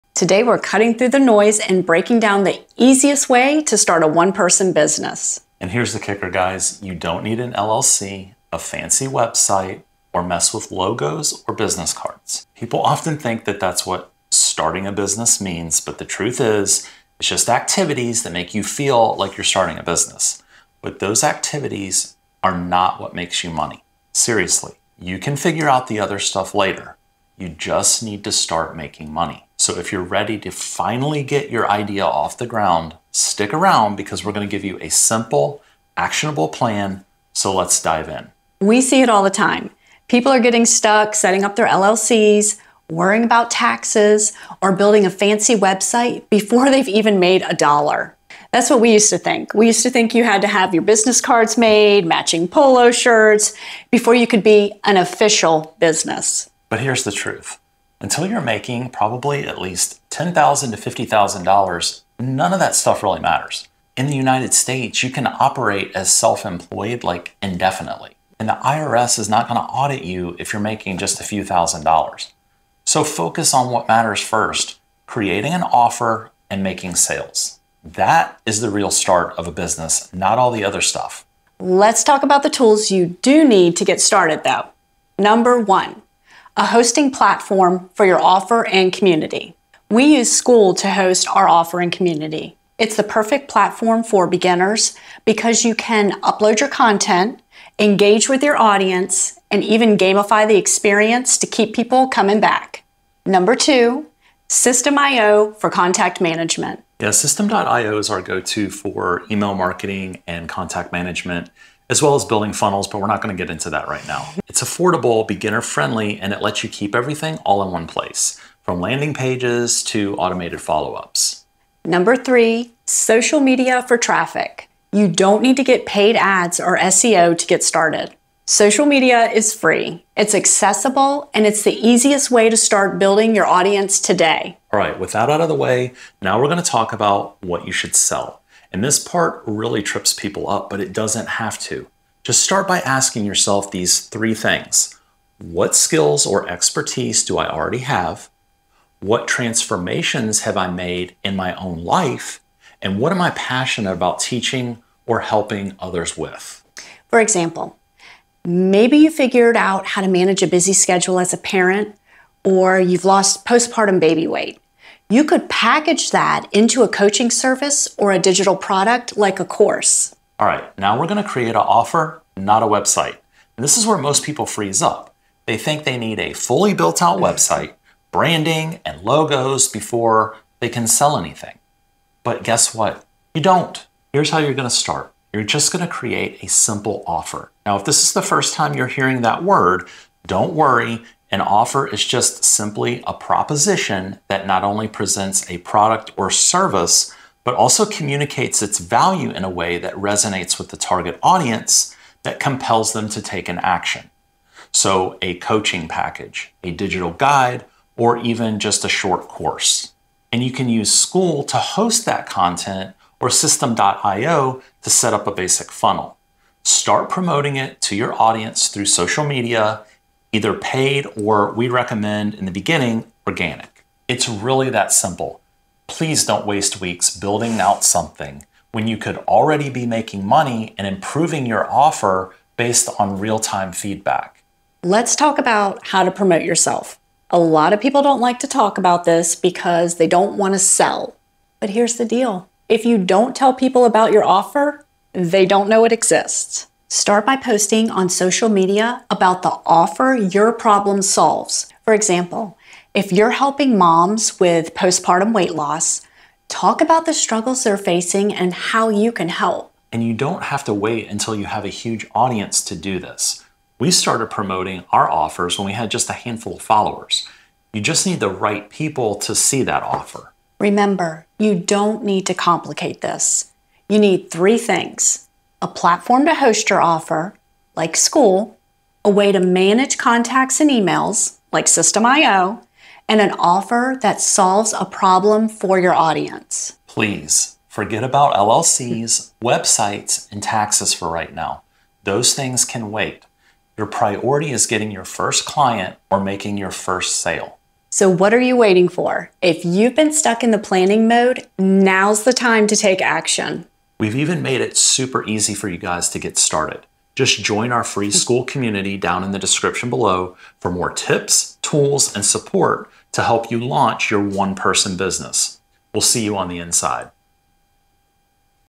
EASIEST One Person Business to Start Making Money Online in 2025
JOIN OUR FREE SKOOL COMMUNITY👇 Let's build a business together!

Ways to Work with Us

About:
Are you overcomplicating the process of starting your business? In this video, we break down the easiest way to start a one-person business in 2025—without worrying about LLCs, websites, or fancy tools.

We’ll show you how to:
✔️ Create an offer using your skills, passions, or transformations.
✔️ Use tools like Skool and Systeme.io to host your course, manage emails, and accept payments.
✔️ Start making money and building momentum, even with zero followers.

Forget the overwhelm. Focus on what matters: solving problems and earning income. Ready to take action? Watch now and let’s get started!